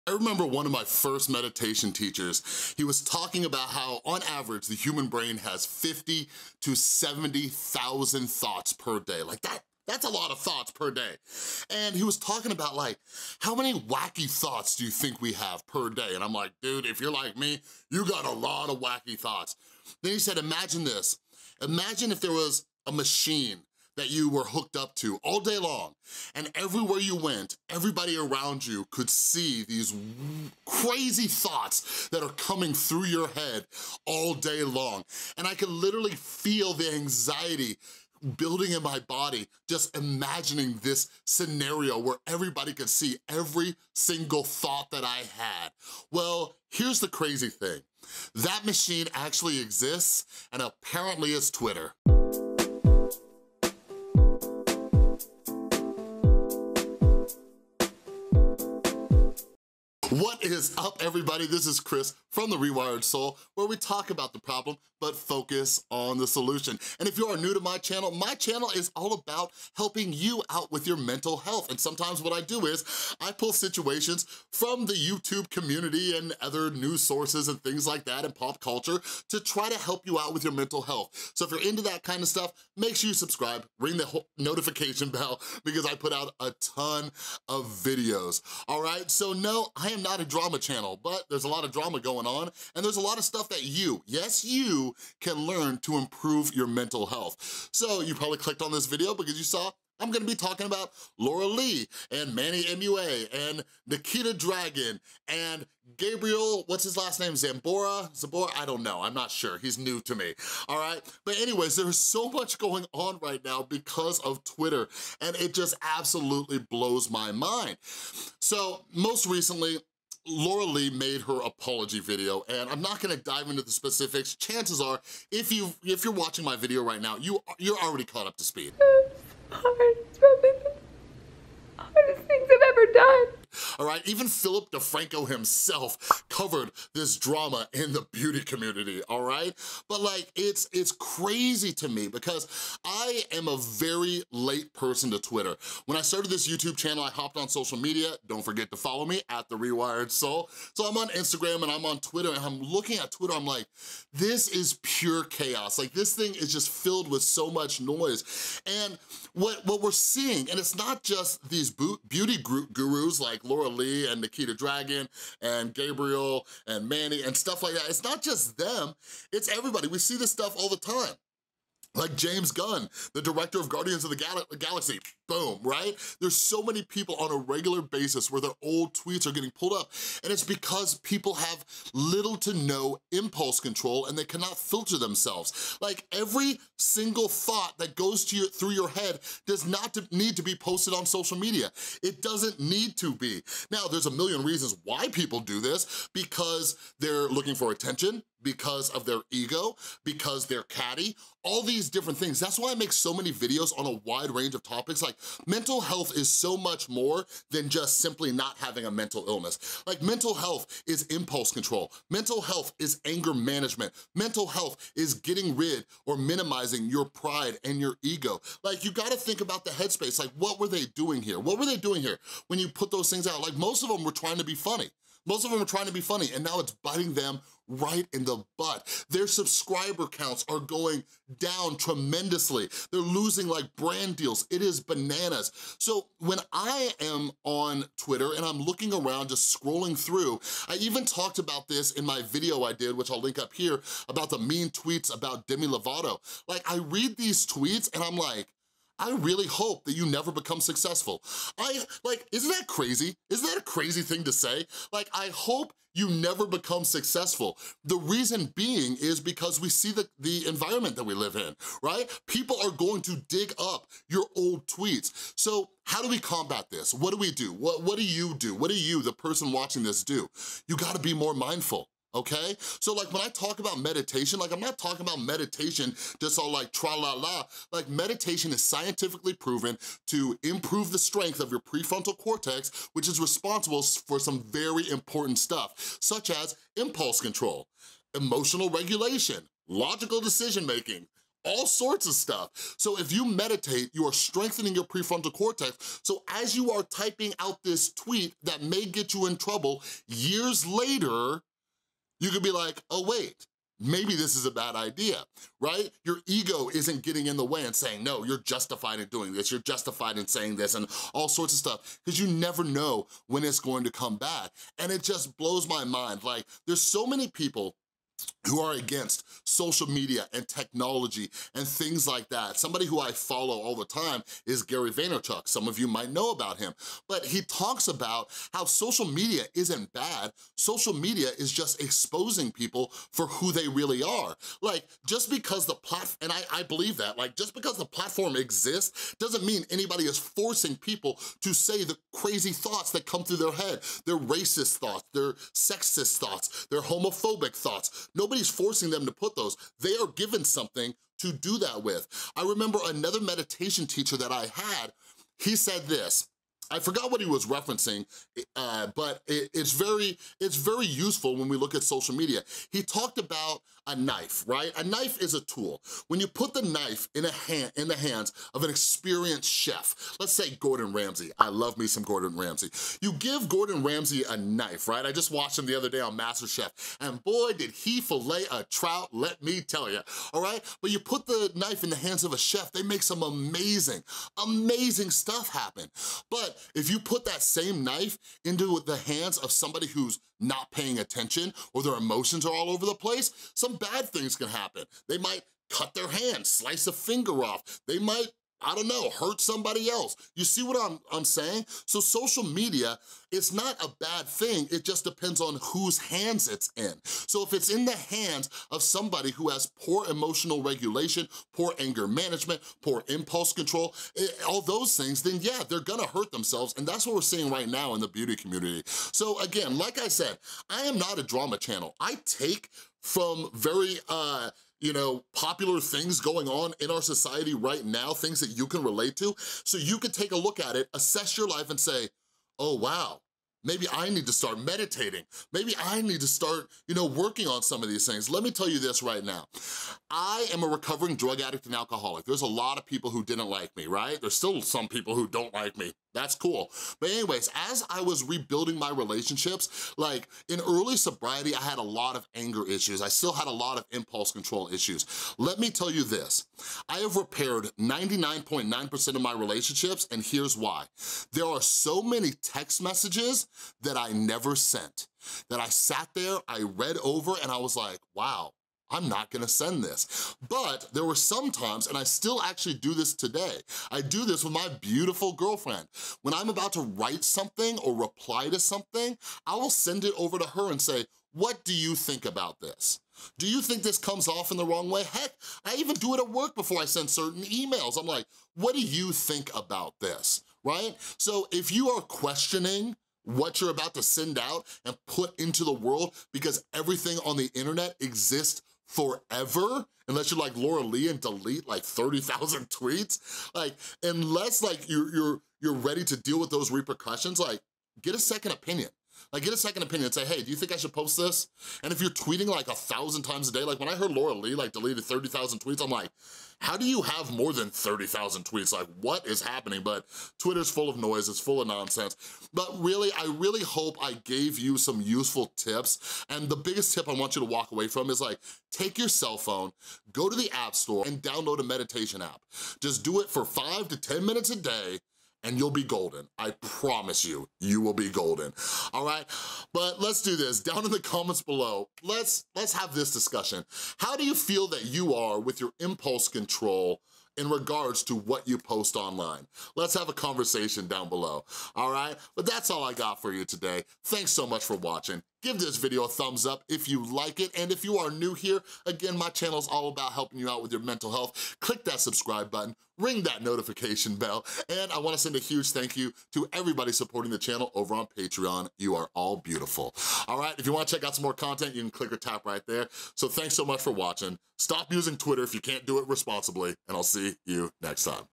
GET OFF TWITTER LAURA LEE (Apology Reaction After Drama w/ Gabriel Zamora vs Jeffree Star)
The Laura Lee apology has come after the bad reaction to her tweets that include racism from years ago. This all started with a picture tweeted by Gabriel Zamora, Manny MUA, Nikita Dragun and Laura, which then led to some mean tweets about Jeffree Star. The beauty community has blown up with all of this drama, and Manny ended up getting outted for a face he made at a meet and greet to a fan named Dan from the UK. In a new video, Manny apologizes and explains what happened, and that's when Laura Lee made her video. Her subscriber count is now dropping rapidly, and there's something we can learn from it.